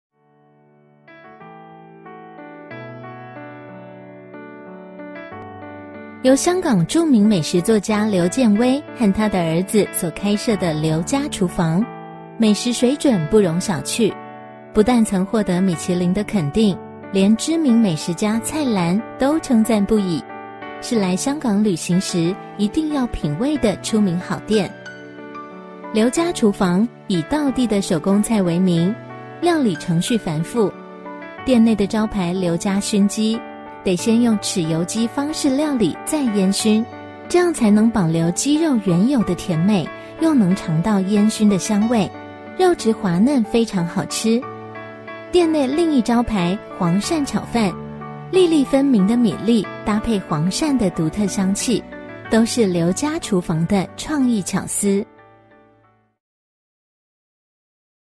[香港] 劉家廚房 (美食)
由香港著名美食作家劉健威和他兒子所開設的劉家廚房，美食水準不容小覷，不但曾獲得米其林的肯定，連知名美食家蔡瀾都稱讚不已，是來香港旅行時一定要品味的出名好店！劉家廚房以道地的手工菜為名，料理程序繁複，店內的招牌留家燻雞，得先用豉油雞方式料理再煙薰，這樣才能保留雞肉原有的甜美，又能嚐到煙燻的香味，肉質滑嫩非常好吃。店內另一招牌黃鱔炒飯，粒粒分明的米粒，搭配黃鱔的獨特香氣，都是劉家廚房的創意巧思。